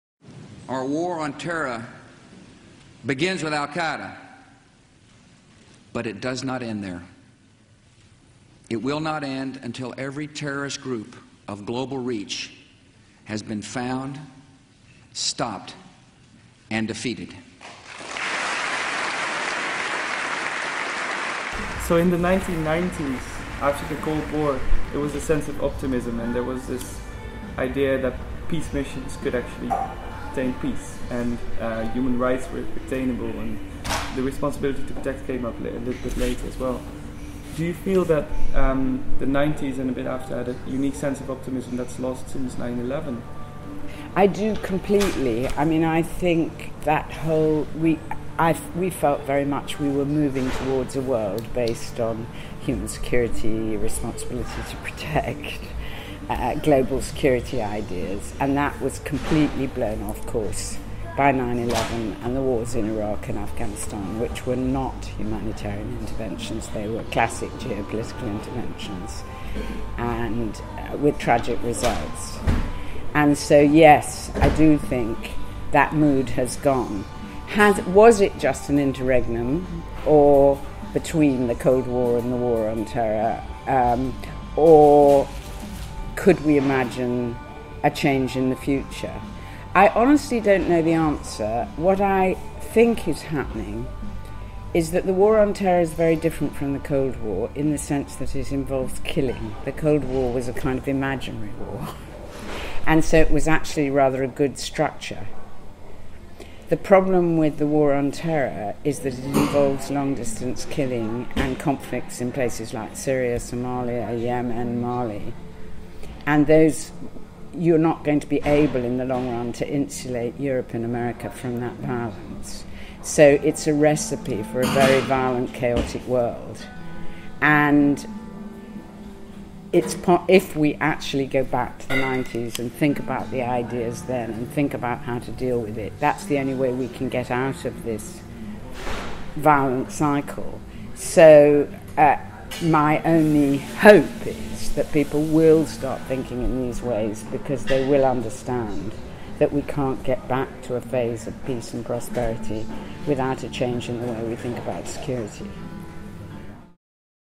Mary Kaldor on the War on Terror
Mary Kaldor (Professor of Global Governance at the London School of Economics) discusses the war on terror and the lost sense of optimism of the 1990s in an interview with War and Peace Talk on 13 June 2013.